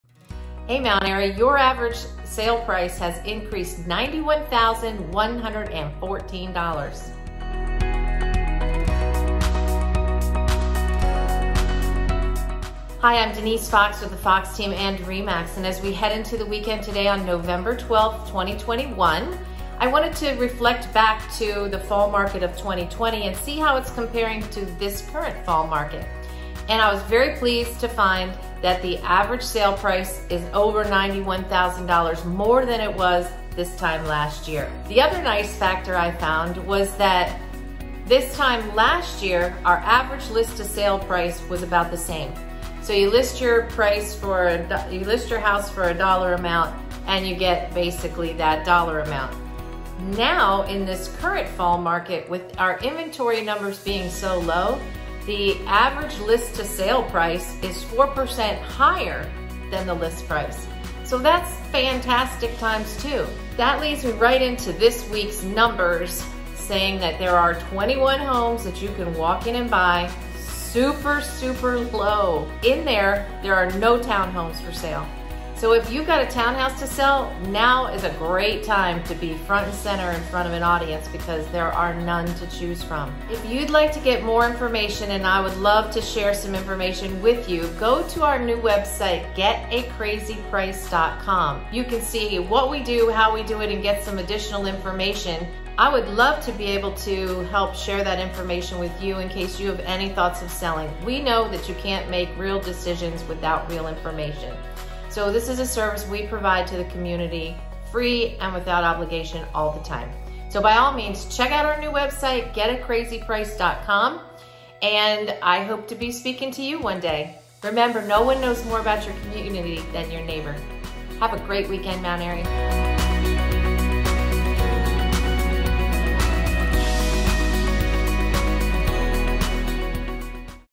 Home values are up over $90k and there are NO townhouses for sale
Mount airy md home values are up $91,114 over this time last year! They are also selling for an average of 4% over list price vs last year where they sold for an aver of $2k under list price   Surprisingly there are also NO townhouses for sale !